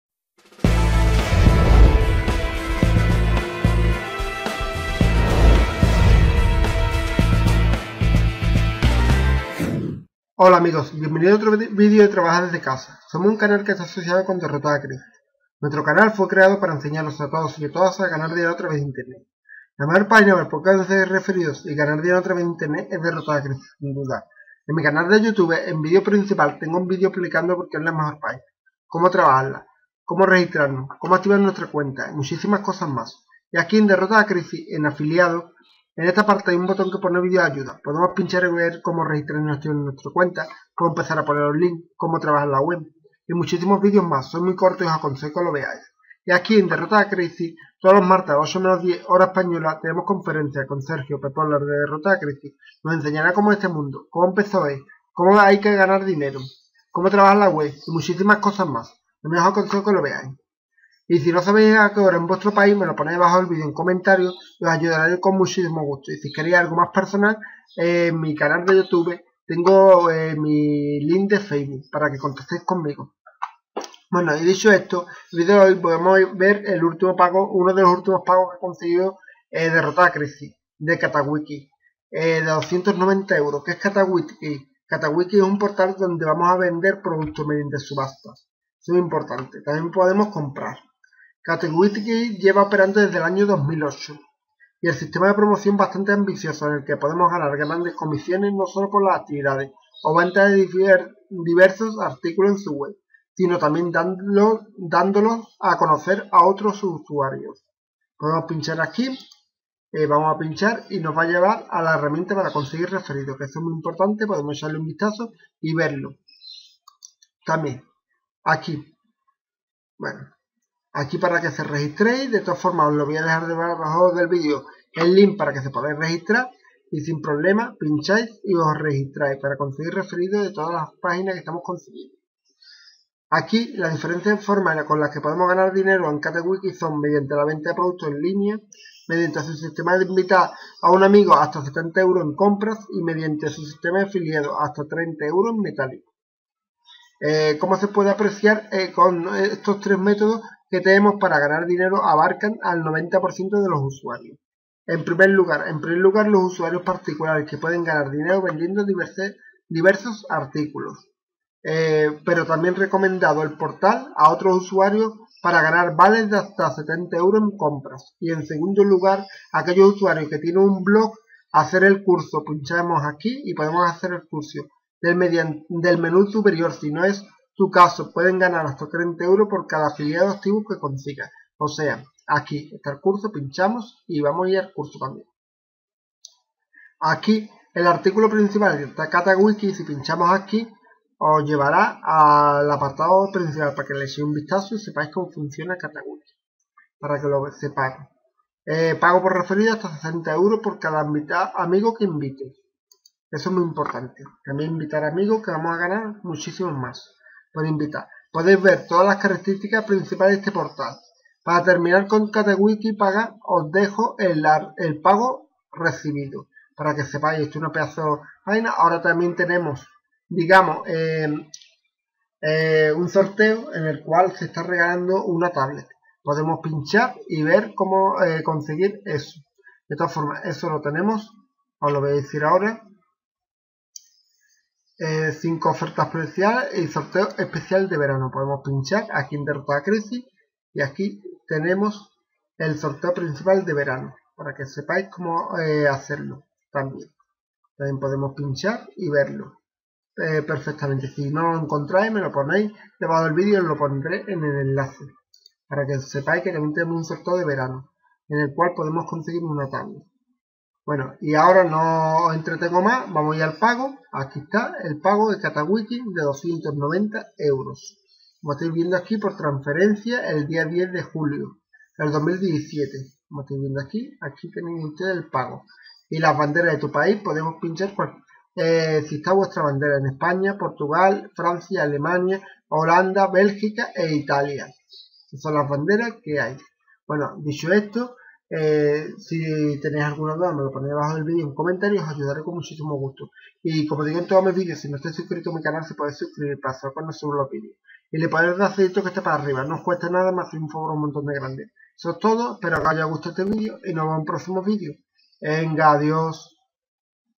CataWiki, Comprobante del Primer Pago 290€ | La Mejor Pagina de Subasta Online | Derrota la Crisis
Hola amigos, en el vídeo de hoy os enseño el Primer Pago Recibido de CataWiki de 290€ por DerrotalaCrisis.

CataWiki es un Portal donde se VendenArticulos mediante Subastas.

CataWiki lleva operando desde el año 2008, donde tiene un sistema donde podemos Ganar Grandes comisiones no solo por la actividad o Venta de diversos artículos en su Web, si no dándolo a conocer a otros usuarios.